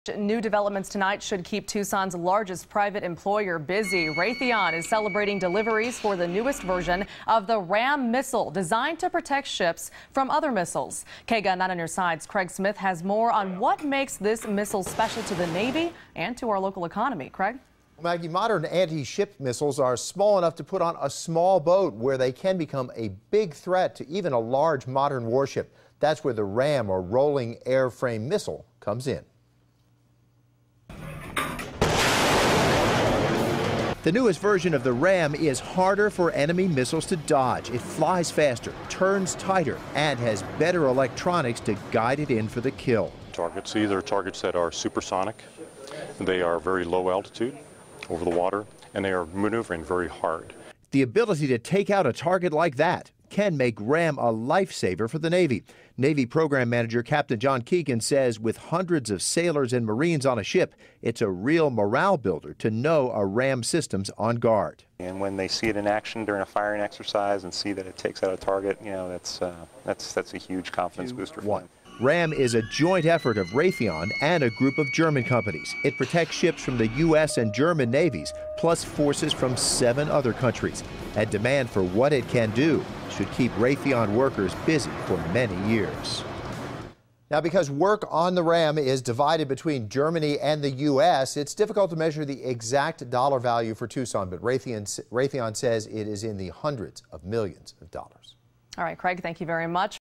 Raytheon delivers upgraded ship-protection missile
The RAM, or Rolling Airframe Missile is designed to protect warships from anti-ship missiles